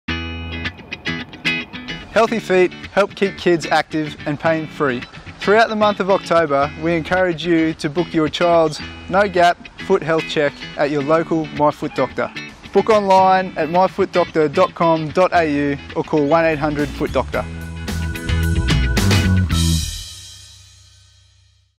My FootDr Foot Health Month – NO GAP Kids Foot Health Checks
Healthy feet help keep kids active and pain free. Throughout October, we encourage you to book your child’s NO GAP foot health check and keep children on their feet.

or call 1800 FOOTDR.